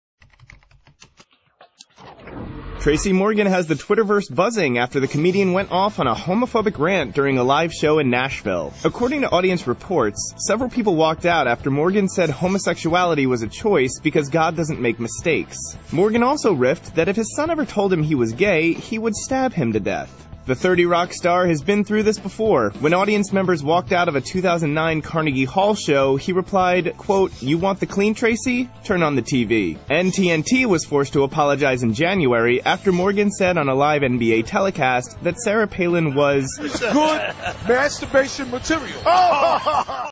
Tracy Morgan's Homophobic Rant; 30 Rock Star Under Fire Following Stand-up Show in Nashville
Tracy Morgan has the Twitterverse buzzing after the comedian went off on a homophobic rant during a live show in Nashville. According to audience reports, several people walked out after Morgan said homosexuality was a choice because god doesn't make mistakes. ; Morgan also riffed that if his son ever told him he was gay, he would stab him to death. The 30 Rock star has been through this before.